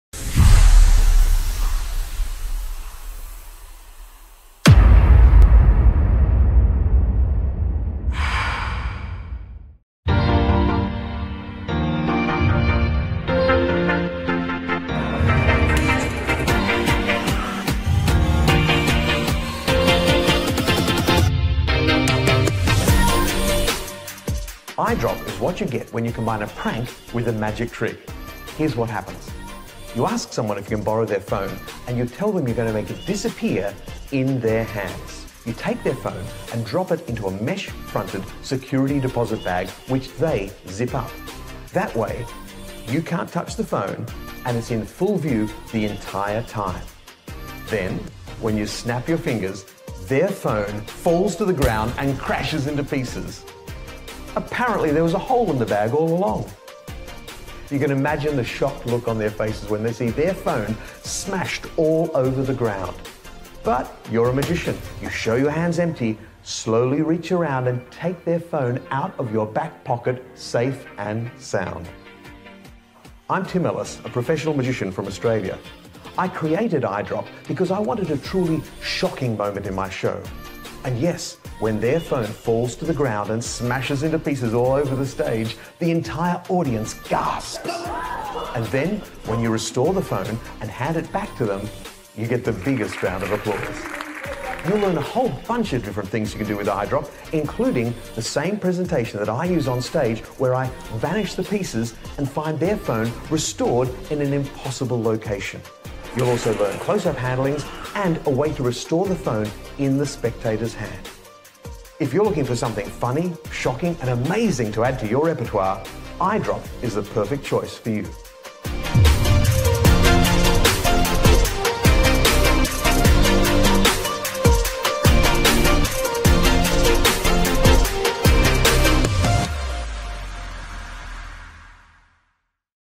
iDrop porTim Ellis
iDrop es a la vez un artículo de broma increíblemente divertido Y el sueño de un artista de teatro. Coloca un teléfono prestado en una bolsa de malla transparente y ciérralo para protegerlo. Mientras es sostenido por un espectador, el teléfono celular "accidentalmente" se cae de la bolsa y se rompe en pedazos en el suelo. Tienes el control secreto del teléfono prestado y puedes revelarlo donde quieras.

Si lo juegas como una broma, simplemente puedes sacar el teléfono de tu bolsillo y devolvérselo. iDrop nunca deja de provocar GRITOS cuando el espectador cree que se ha caído y destruido su propio teléfono. Pero también puedes revelar el teléfono en un lugar imposible: envuelto en un regalo, en un nido de cajas o sellado en un frasco. El lugar donde revela el teléfono prestado está limitado solo por su imaginación.

iDrop es una bolsa secreta que cambia un teléfono prestado por un señuelo. El señuelo está modelado en tamaño, peso y apariencia a un iPhone o un diseño similar de teléfono celular. Cuando golpea el suelo, convincentemente se "rompe" en varios pedazos, pero se vuelve a ensamblar y reajustar fácilmente para cada uso. Cuando te deshagas de la bolsa en iDrop, puedes aprovechar esta oportunidad para cargar secretamente el teléfono prestado donde quieras, o meterlo en tu bolsillo trasero para devolvérselo al espectador.

El paquete incluye: bolso especialmente trucado, teléfono rompible e instrucciones en línea.